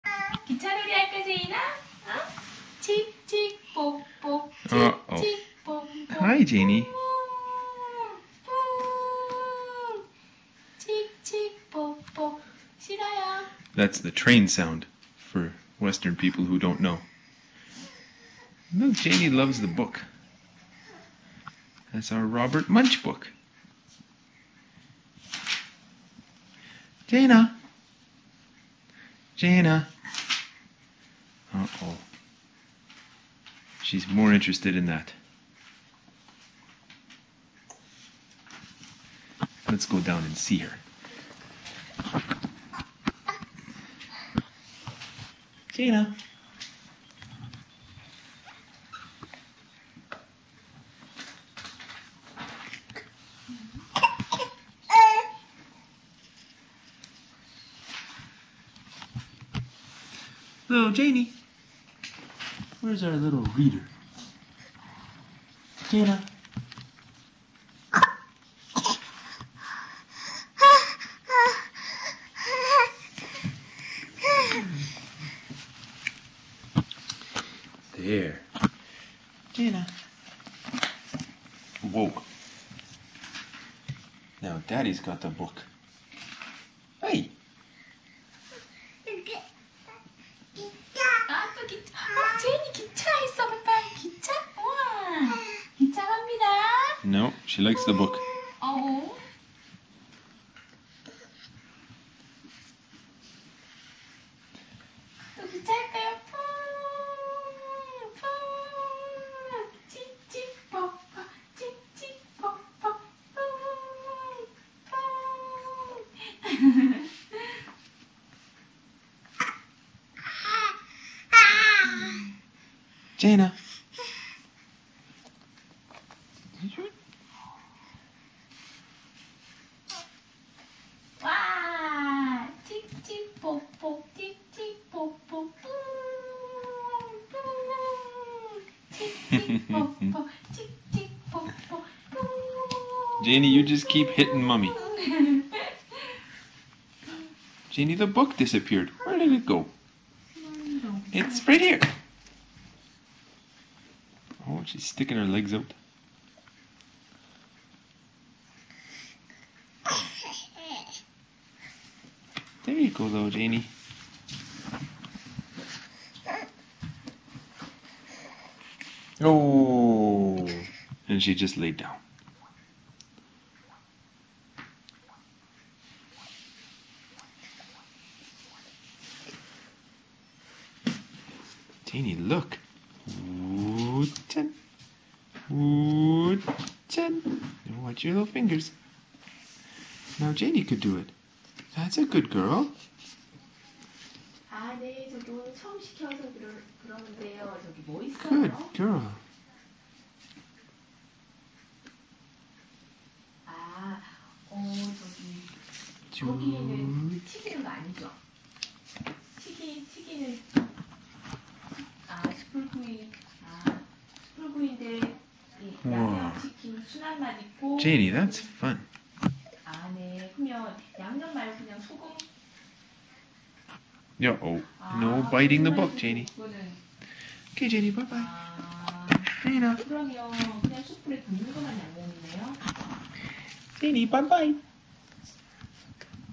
Jayene 8 Months 20 Days - 2
Jayene can't decide whether to play train with Mommy or read a book with Daddy.